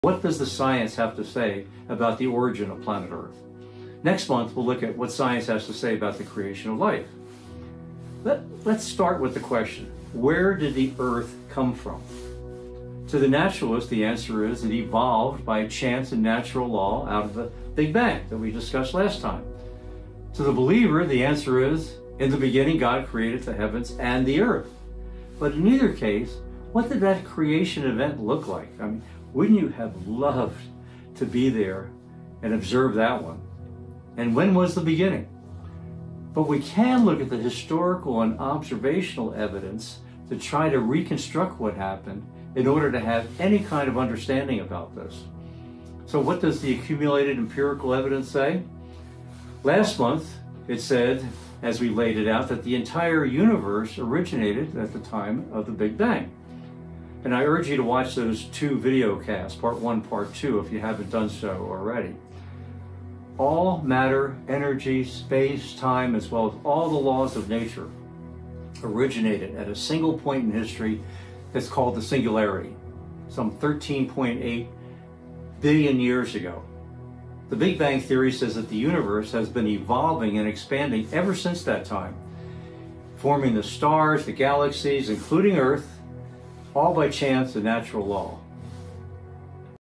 Origin of Earth: What Does Science Say?
Where did planet Earth come from?
In this short clip, we explore what science says about the origin of our world. From the Big Bang to natural law, we contrast the naturalist and believer perspectives while asking deeper questions: What did that creation event look like? When did it happen?
This is part of a broader series diving into science, faith, and the origins of life.
Watch, reflect, and join the conversation.